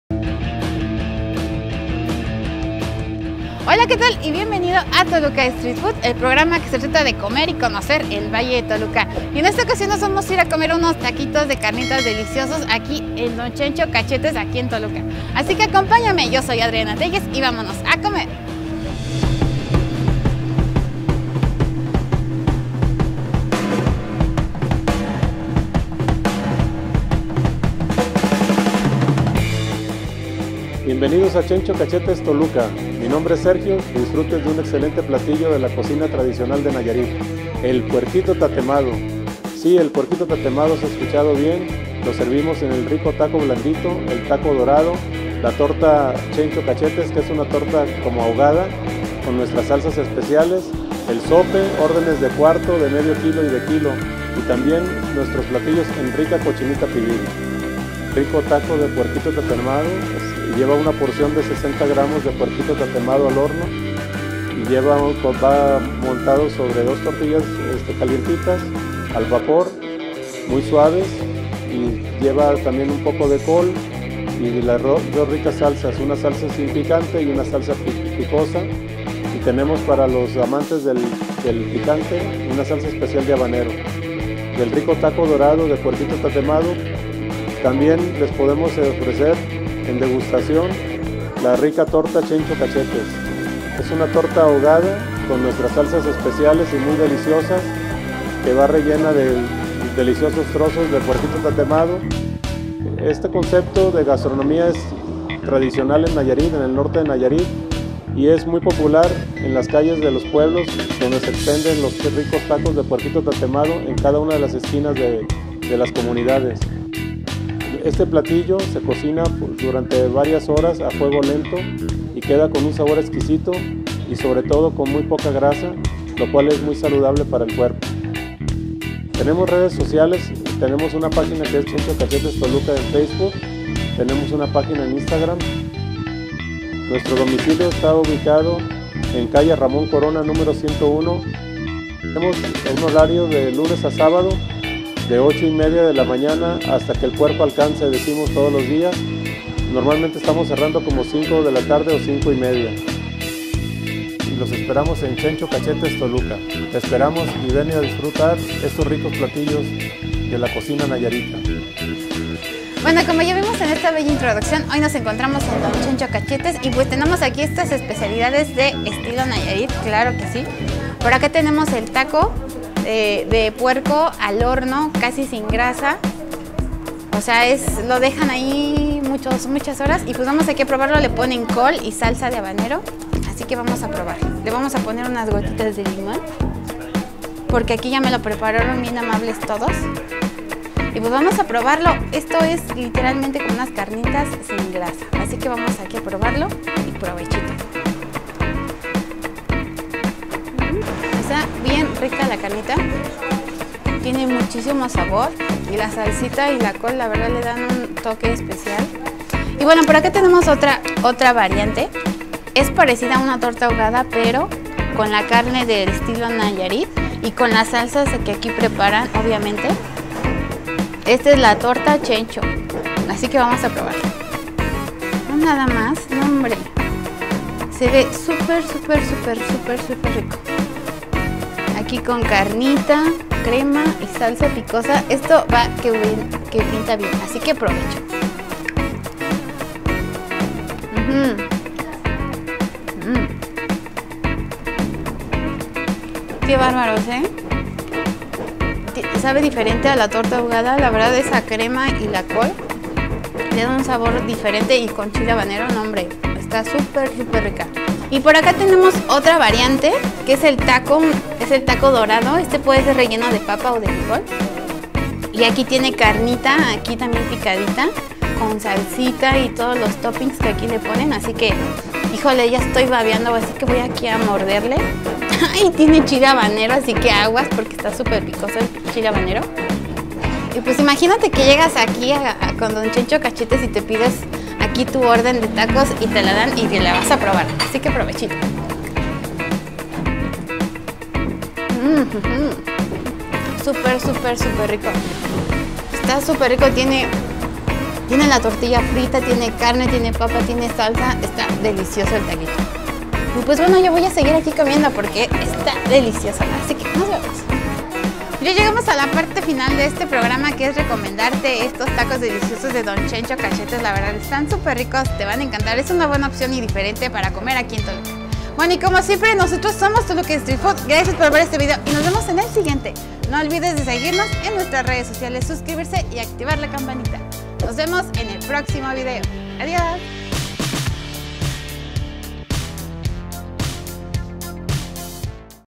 Tacos Chencho Cachetes TOLUCA STREET FOOD
PUERQUITO TATEMADO, DE NAYARIT PARA EL MUNDO, NO TE PIERDAS ESTA EXPERIENCIA CULINARIA.EN CHENCHO CACHETES TOLUCA UN SABOR DIFERENTE Y CERO GRASOSO, CONOCELO!